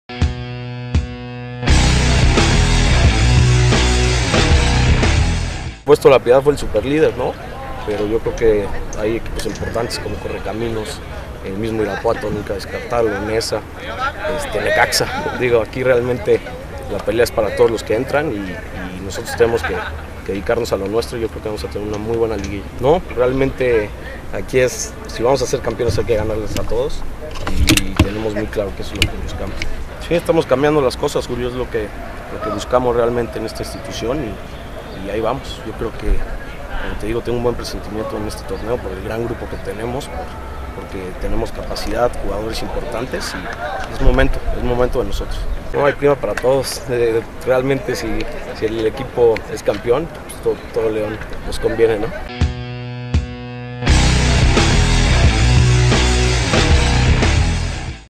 Jesús Martínez Presidente del Club León opina sobre La Liguilla
Jesús Martínez Presidente del Club León opina sobre La Liguilla. La afición de Milenio Televisión León